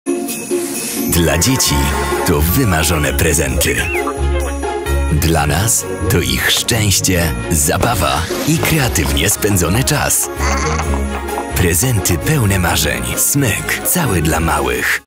Prezenty pełne marzeń! Znajdziesz je w SMYKU!
Premiera świątecznej kolekcji w sklepach SMYK i na smyk.com!

SMYK ma w tym roku wiele propozycji na to, jak spełniając marzenia dzieci, jednocześnie podarować im coś więcej. Zaczynając od dostępnych tylko w SMYKU zabawek SMIKI, przez kultowe LEGO, cieszące się niesłabnącą popularnością Squishmallows, Barbie, Rainbow High czy Hot Wheels, aż do drewnianych zabawek Hape i jednej z niewielu zabawek naprawdę kochanych przez nastolatków – Nerfa. W tegorocznej kampanii prezentujemy wiele innowacyjnych zabawek – jak rozkładane silikonowe pianinko SMIKI, multifunkcyjny robot czy też interaktywna owieczka Little Live Pets.
Prezenty pełne dziecięcych marzeń znajdziecie w SMYKU.